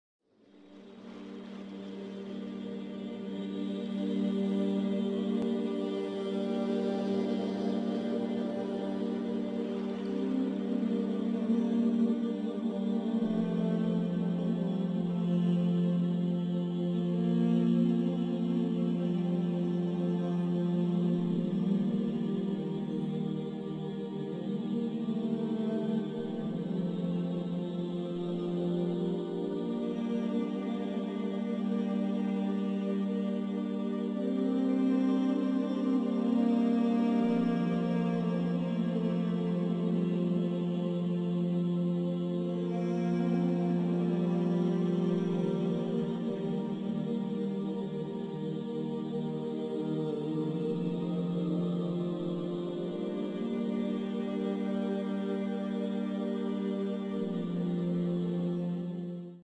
Say, ‘God! Creator of the heavens and earth! Knower of all that is hidden and all that is open,
Say, ‘God! Creator of the heavens and earth! Knower of all that is hidden and all that is open, You will judge between Your servants regarding their differences.’ — THE THRONGS (az-Zumar) 39:46

المصباح‎‎
Watch, read, reflect & share